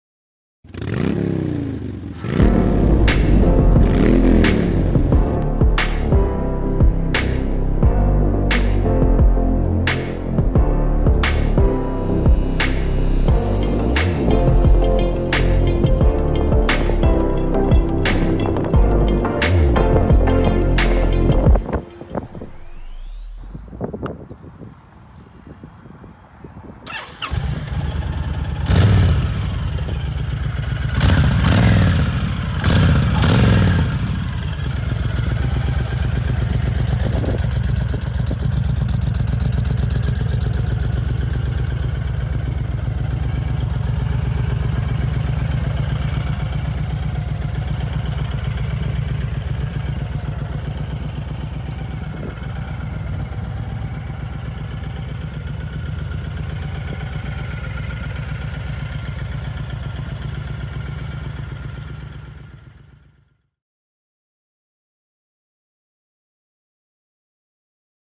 2013 HONDA FURY
This 2013 Honda Fury runs, drives and looks like new. This bike is incredibly confortable, excellent cruiser bike, very reliable motorcycle, chrome and paint are great, no dents. Powered by a 1312 cc V-twin engine. Great torque. Very low 945 miles, clean title.
VIN# JH2SC61N9DK101312
We finance//We trade//We buy motorcycles//Hablamos español**30 DAYS WARRANTY**